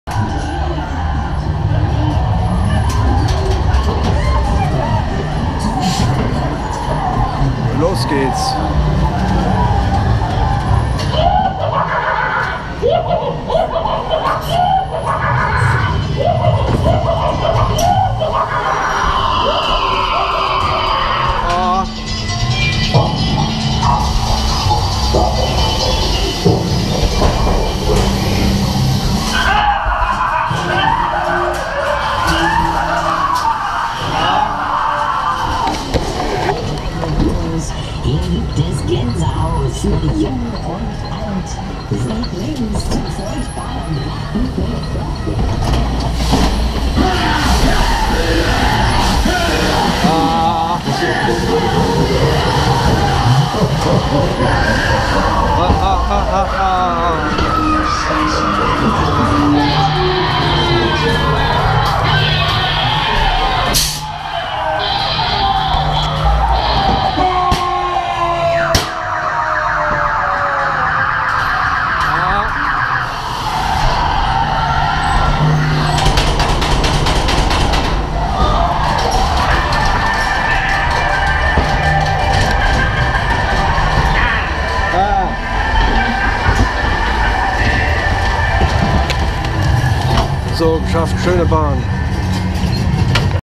Geister Villa - Molengraft / Sipkema (Onride) Video von der Pforzheimer Messe 2023
Hersteller: Barbisan
Typ: 2 Etagen Geisterbahn

Fahrpreis: 5€

Geister Villa von Molrngraft / Sipkema POV Onride Video von der Messe in Pforzheim 2023 / Kirmes